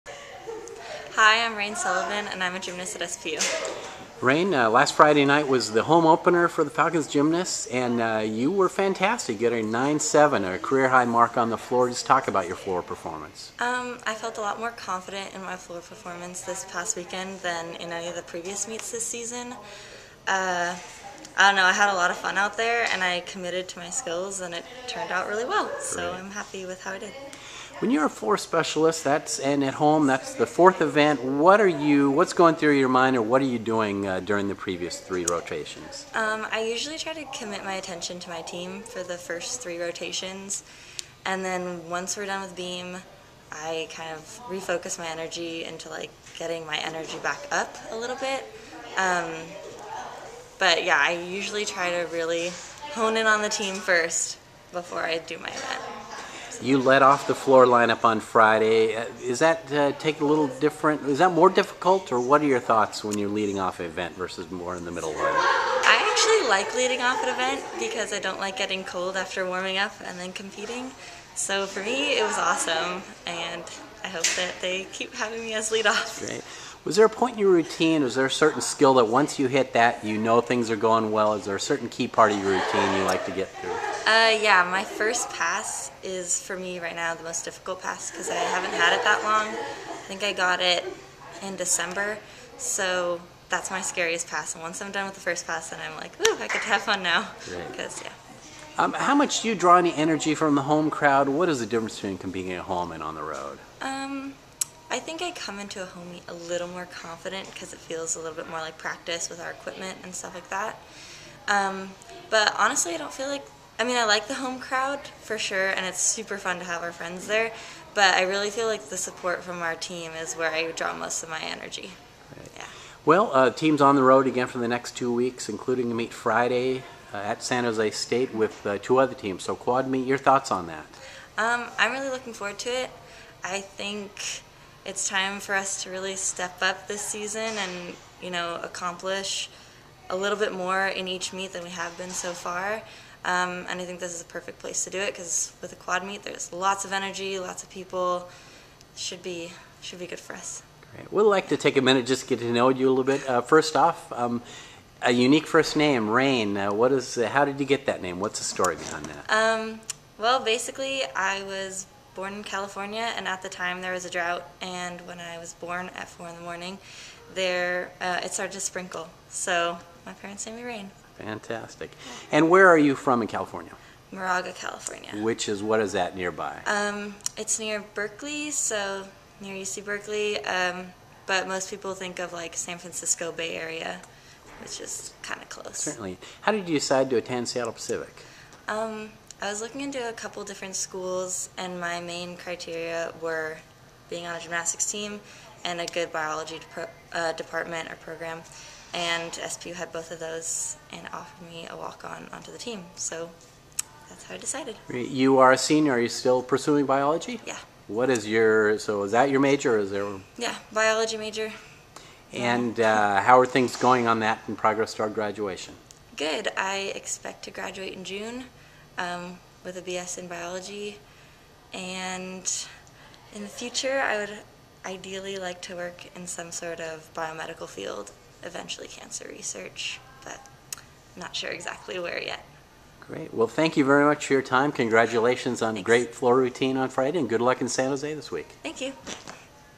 SPU GYMNASTICS: Rain Sullivan (Jan. 25, 2016)
Seattle Pacific senior gymnast Rain Sullivan talks about Friday's career-best 9.70 floor routine, her unique first name and about her major.